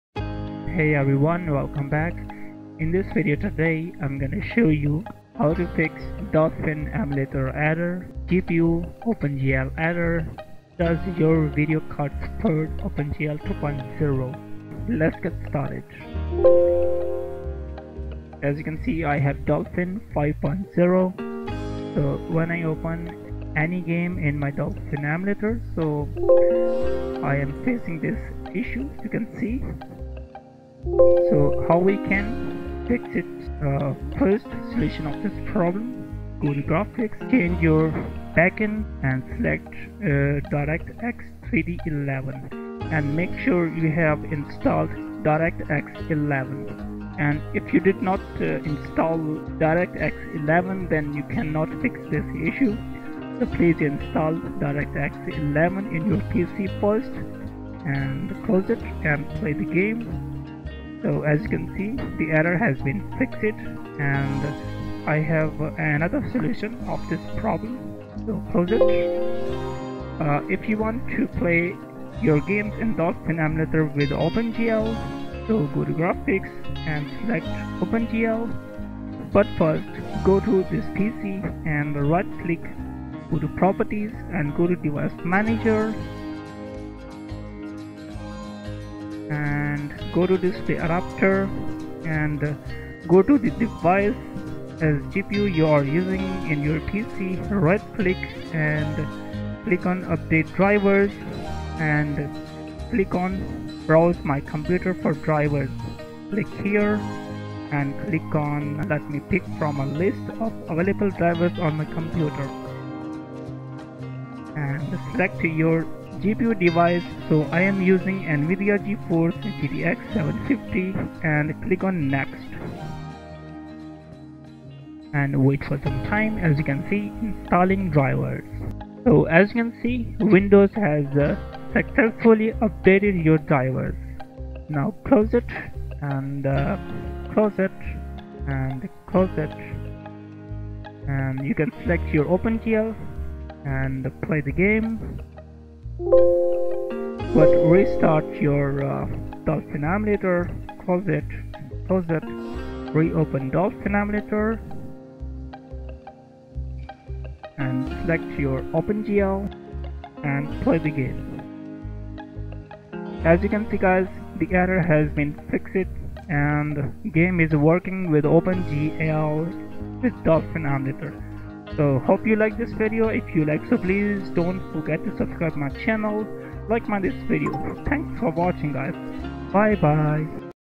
Dolphin GPU : OGL Error Does your video card support OpenGL 2.0
Hi,
I have showed in this video how can you fix dolphin GPU : OGL error Does your video card support OpenGL 2.0